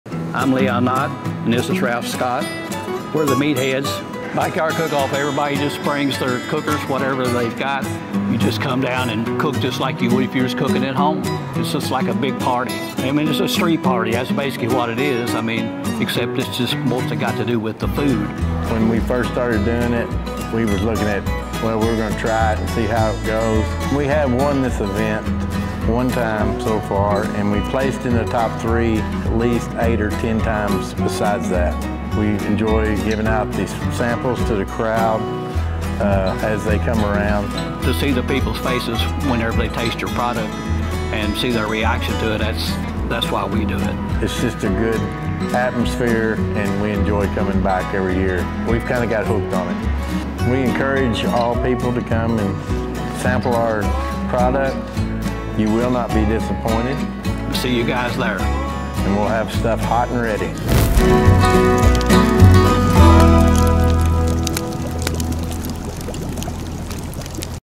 Sign up for the BBQ & Barrels Backyard Cook-off
Do you and your grilling buddies have what it takes to be this year's Backyard Cook Grand Champion? Don't forget to sign up! The registration deadline is April 26.

If grilling isn't your thing - make sure to walk down 2nd street Saturday, May 11 and enjoy all the BBQ samples offered from the various teams. It's one of the MANY ways you can enjoy your favorite BBQ at BBQandBarrels.

Sign up for the cook-off and find more event information at BBQandBarrels.com